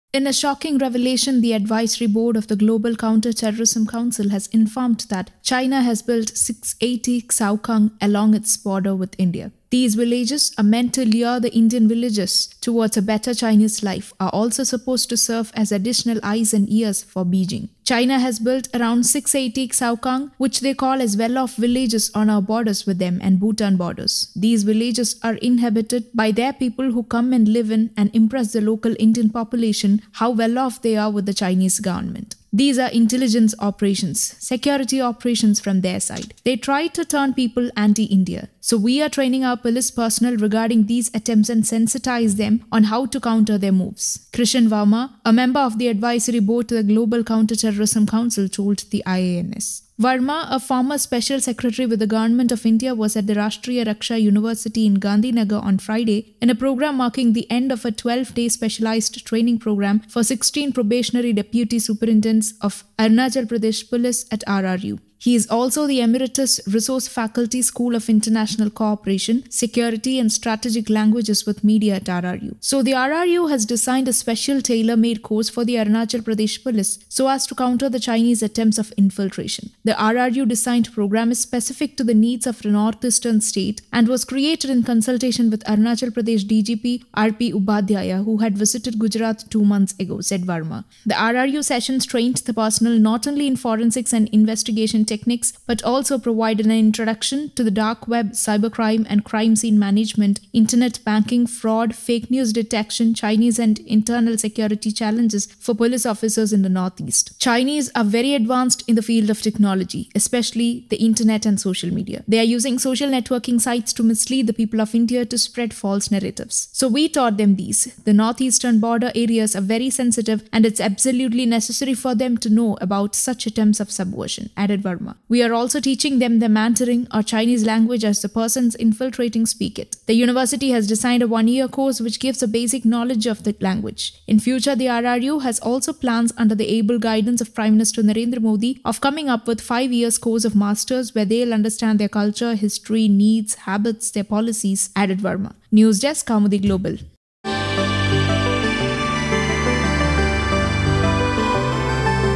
680 Chinese villages thriving near the Indian borders ‘try to turn people anti-India’: Expert
In a shocking revelation, the Advisory Board of the Global Counter Terrorism Council has informed that China has built 680 'Xiaokang' (prosperous or thriving villages) along its border with India.
These villages are meant to lure the Indian villagers towards a better Chinese life, are also supposed to serve as additional eyes and ears for Beijing.
"China has built around 680 Xiaokang, which they call as 'well off' villages on our borders with them, and Bhutan borders. These villages are inhabited by their people who come and live in and impress the local Indian population how well off they are with the Chinese government.
"These are intelligence operations, security operations from their side. They try to 'turn people anti-India'. So we are training our police personnel, regarding these attempts and sensitize them on how to counter their moves," Krishan Varma, a member of the Advisory Board to the Global Counter Terrorism Council told the IANS.
Varma, a former special secretary with the Government of India (GoI), was at the Rashtriya Raksha University (RRU) in Gandhinagar on Friday in a program marking the end of a 12-day specialised training programme for 16 probationary Deputy Superintendents (DySP) of Arunachal Pradesh police at RRU. He's also the Emeritus Resource Faculty, School of International Cooperation, Security and Strategic Languages with Media at RRU.
"So the RRU has designed a special tailor-made course for the Arunachal Pradesh police, so as to counter the Chinese attempts of infiltration. The RRU designed program is specific to the needs of the northeastern state and was created in consultation with Arunachal Pradesh DGP RP Upadhyaya, who had visited Gujarat two months ago," said Varma.
The RRU sessions trained the personnel not only in forensics and investigation techniques but also provided an introduction to the dark web, cybercrime and crime scene management, internet banking, fraud, fake news detection, Chinese and internal security challenges for police officers in the northeast.
"They (Chinese) are very advanced in the field of technology, especially the internet and social media. They are using social networking sites to mislead the people of India, to spread false narratives. So we taught them these. The northeastern border areas are very sensitive and it's absolutely necessary for them to know about such attempts of subversion," added Varma.
"We are also teaching them the Mandarin (Chinese language) as the persons infiltrating speak it. The university has designed a one0year course which gives a basic knowledge of the language. In future the RRU has also plans, under the able guidance of Prime Minister Narendra Modi, of coming up with five years course of Masters, where they'll understand their culture, history, their needs, habits, their policies," added Varma.
IANS